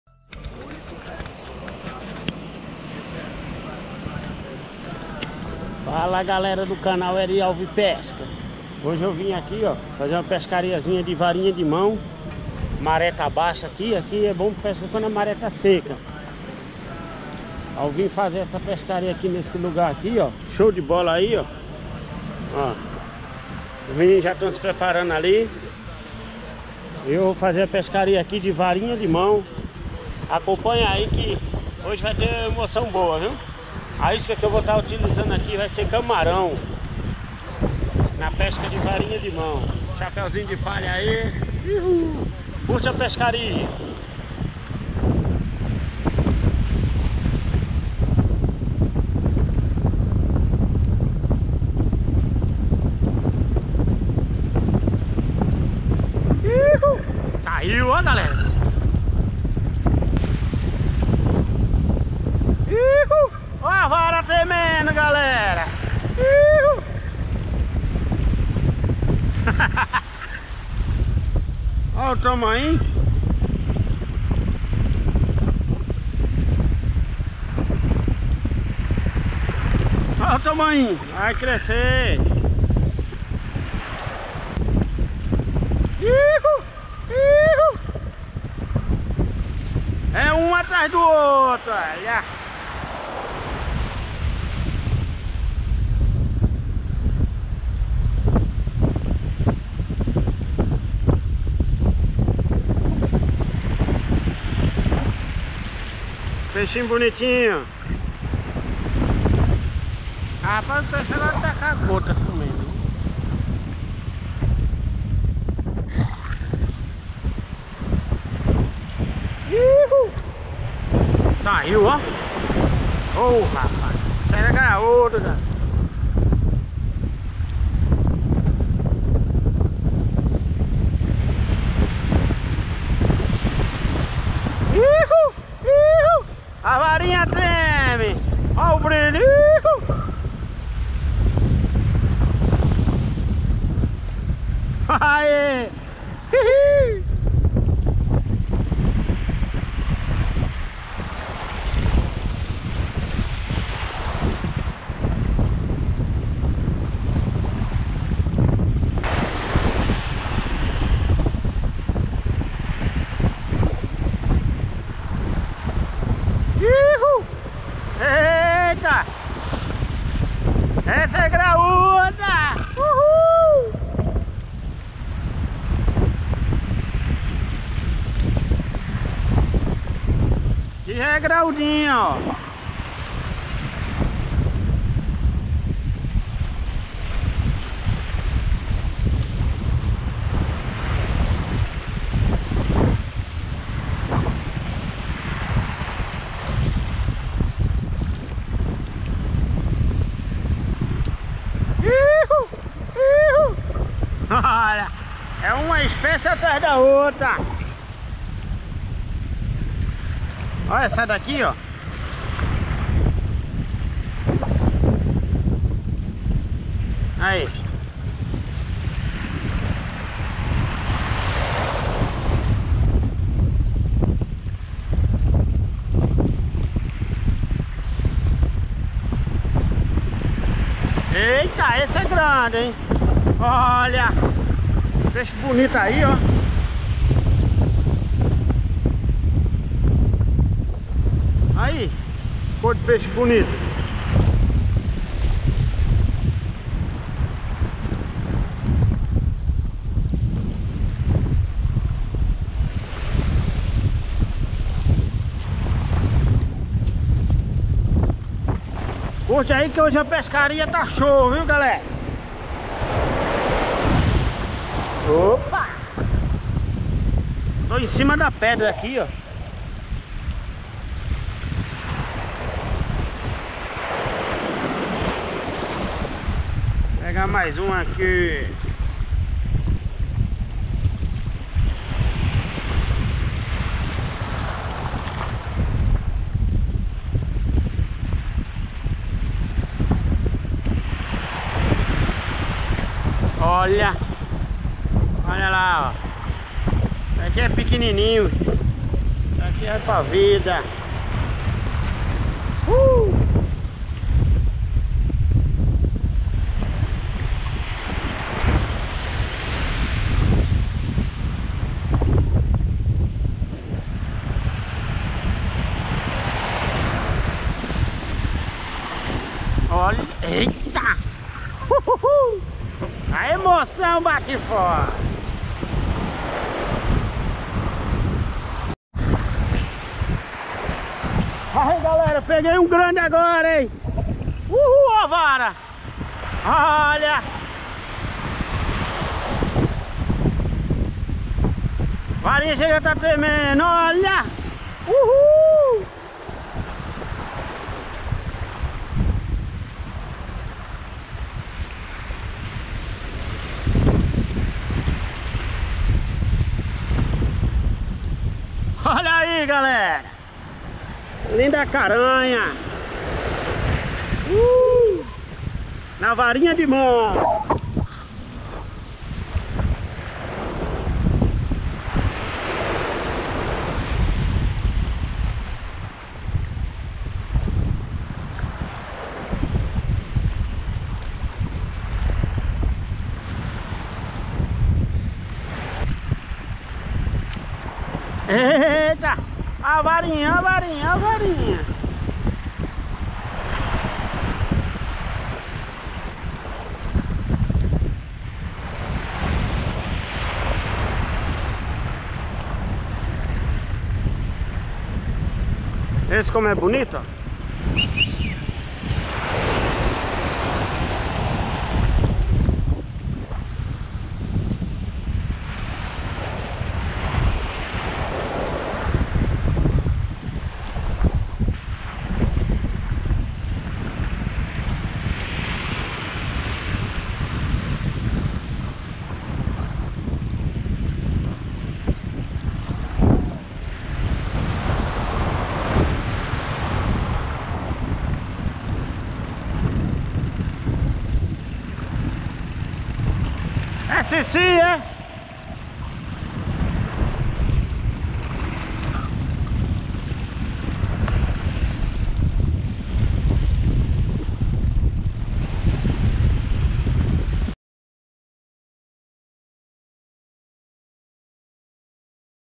PESCA DE PRAIA DE VARINHA DE MÃO, POR QUE SEMPRE NA MARÉ BAIXA? ASSISTA E CONFIRA!
Já se perguntou por que pescar na praia de varinha de mão causa tanta emoção? só que tem um detalhe tem que ser na maré baixa, confira o vídeo até o final e descubra o porque.

PESCA DE PRAIA DE VARINHA DE MÃO, PORQUE SEMPRE NA MARÉ BAIXA? ASSISTA E CONFIRA!